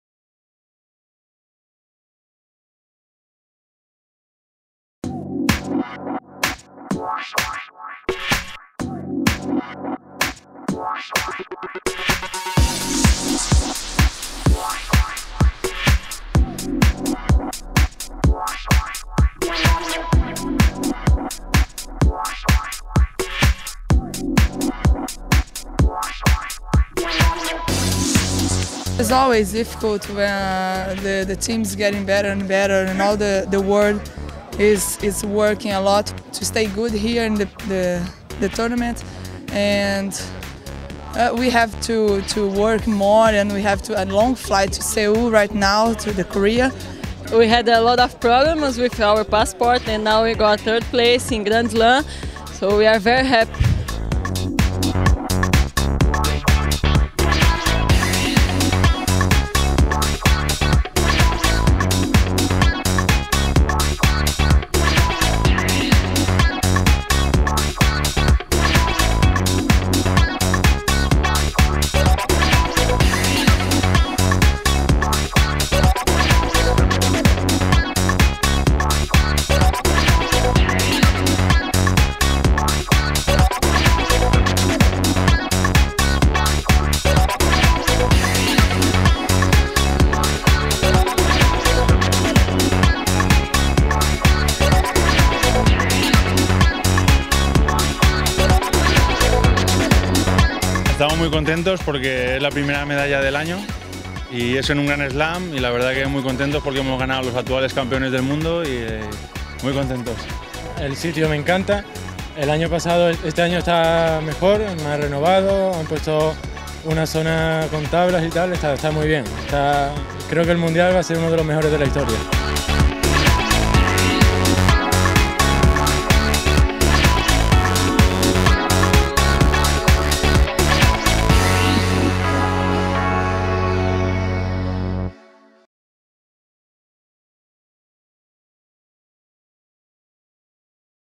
Swatch FIVB World Tour 2010 - L'ultimo gradino del podio
Sale sul podio anche la squadra del Brasile capitanata dalla coppia Antonelli  Talita. Un terzo posto conquistato contro gli americani Branagh  May-Treannor.